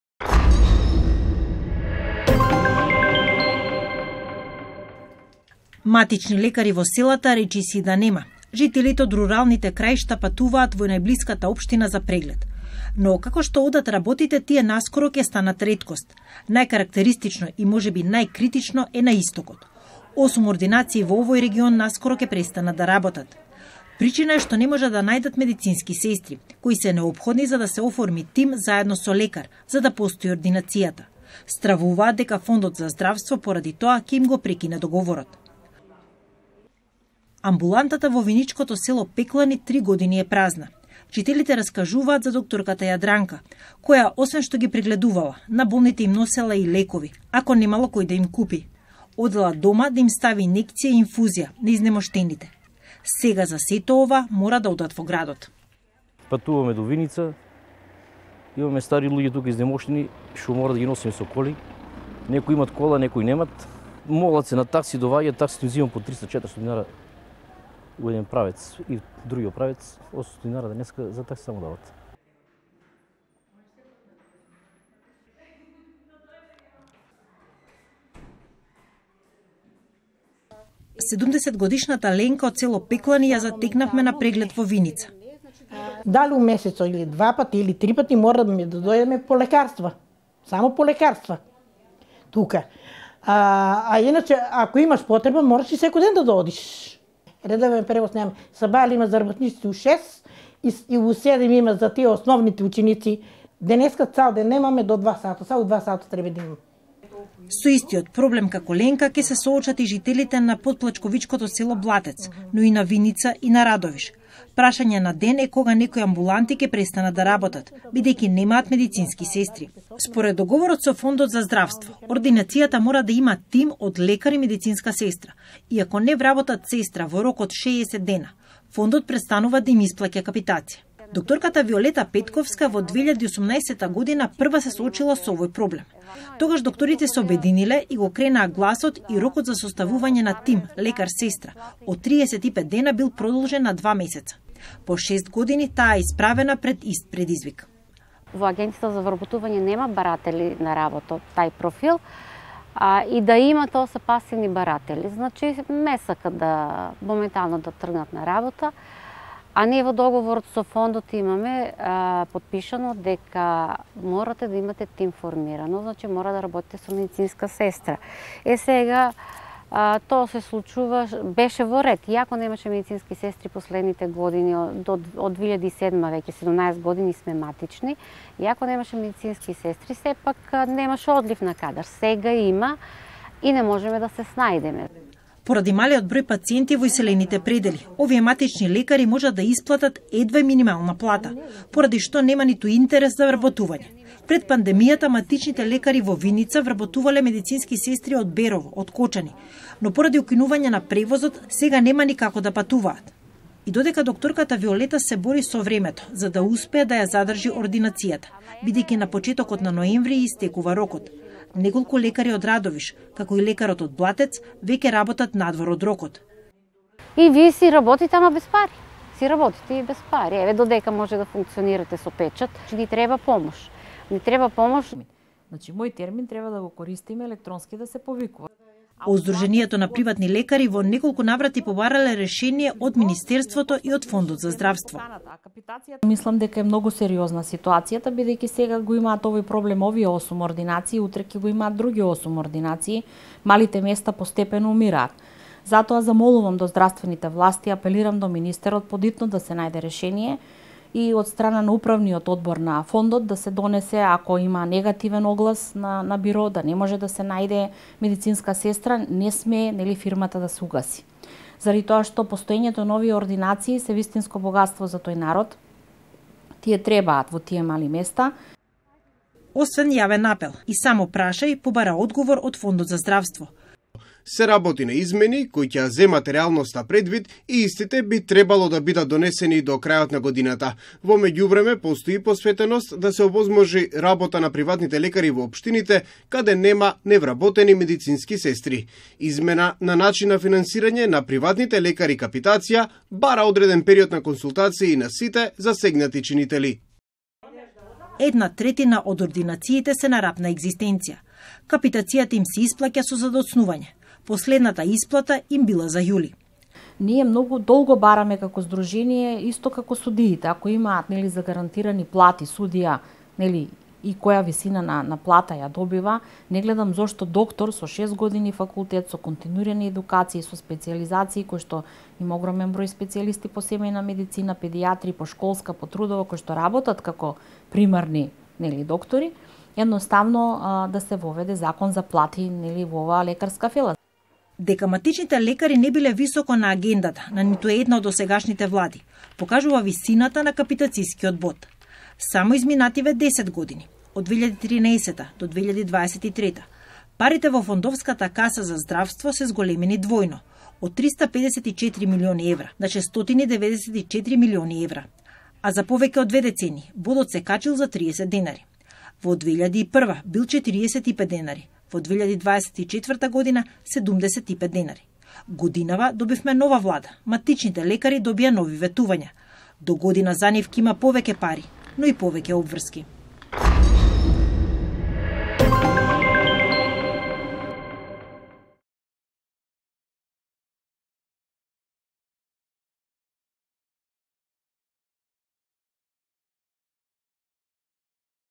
Во селата наскоро ќе нема матични лекари
Матични лекари во селата речиси и да нема. Жителите од руралните краишта патуваат во најбилската општина за преглед, но како што одат работите тие наскоро таму ќе станат реткост. Најкарактеристично и можеби најкритично е на Истокот. Осум ординации во овој регион наскоро ќе престанат да работат. Причина е што не можат да најдат медицински сестри, кои се неопходно за да се оформи тим заедно со лекар за да постои ординацијата. Поради тоа стравуваат дека Фондот за здравство поради тоа ќе им го прекине договорот.